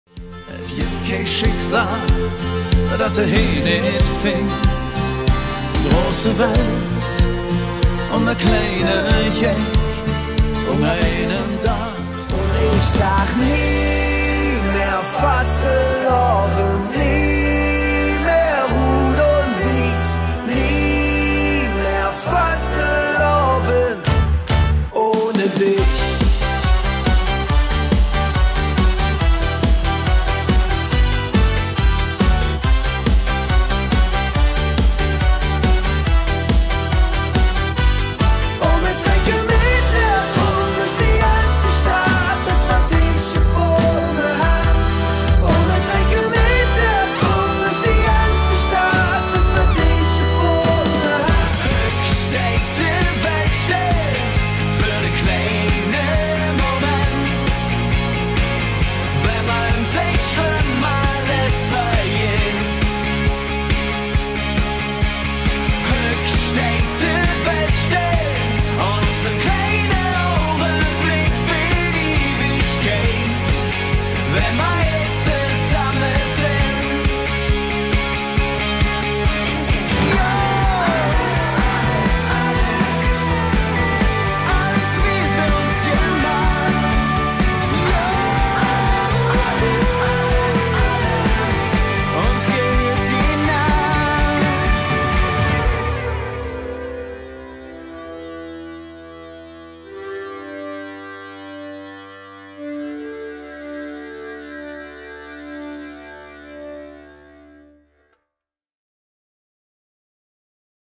Weihnacht lacht – Weihnachtsfeier
Unter dem Motto „Schlemme, Fiere, Danze“ feiert „Weihnacht lacht“ eine jecke Weihnachtsparty. De Räuber, Querbeat und die Domstürmer machen das einzigartige kölsche Lebensgefühl zu einem ganz besonderen Weihnachts-Feier-Erlebnis.